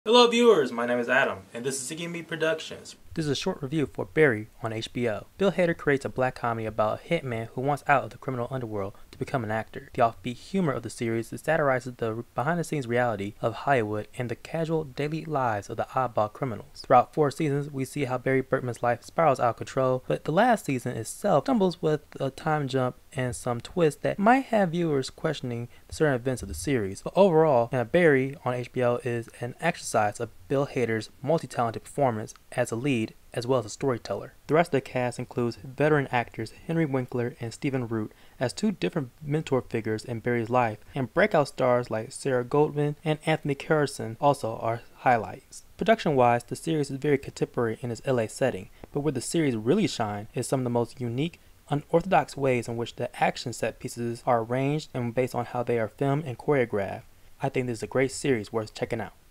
Barry HBO Review
This is a short review of the HBO original series, Barry created by Alec Berg and Bill Hader starring Hader, Stephen Root, Sarah Goldberg, Glenn Fleshler, Anthony Carrigan, Henry Winkler, Sarah Burns, and Robert Wisdom.